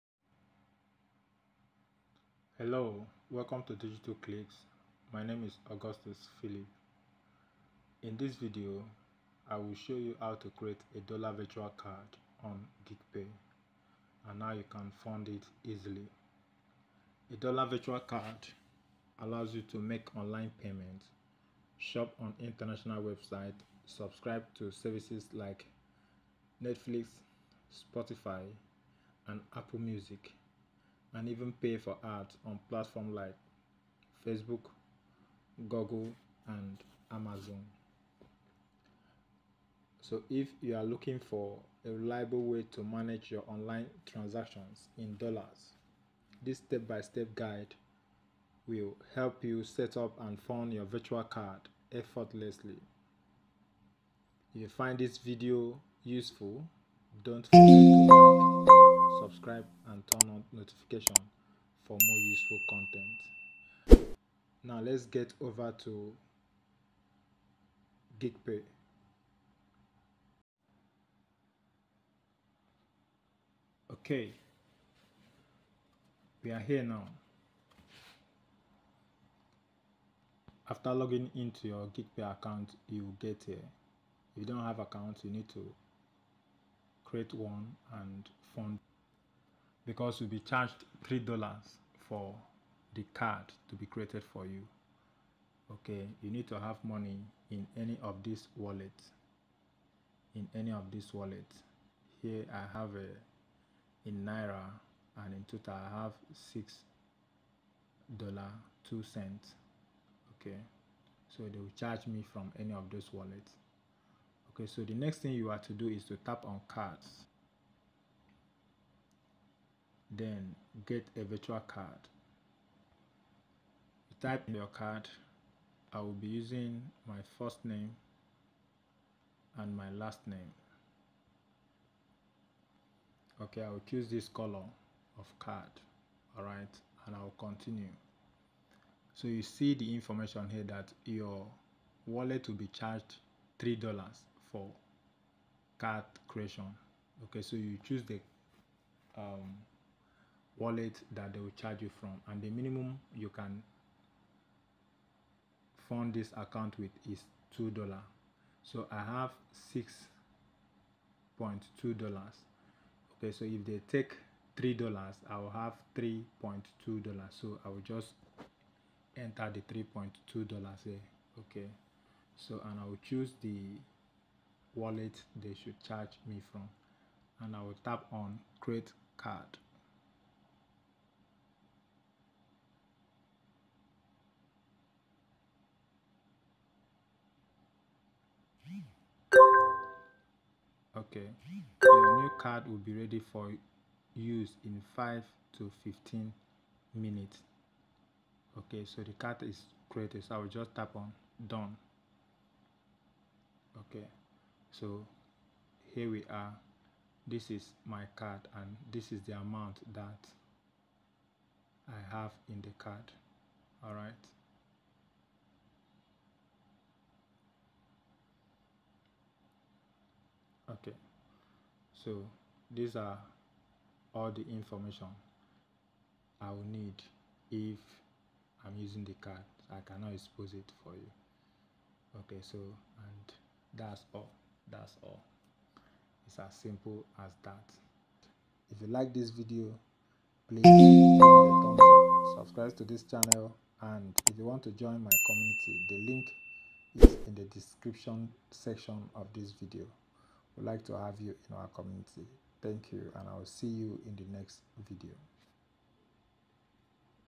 How to Create and Fund Dollar Virtual Card on Geegpay: In Two Simple Steps
📢 How to Create and Fund a Dollar Virtual Card on Geegpay 💳💰
Need a virtual dollar card for online payments, subscriptions, or international transactions? In this video, I'll show you how to create and fund a dollar virtual card on Geegpay step by step. Whether you're a freelancer, online shopper, or business owner, this guide will help you get started quickly and easily!
🔹 What you'll learn in this video:

✅ How to create a dollar virtual card
✅ How to fund your virtual card
✅ Best uses for your Geegpay dollar card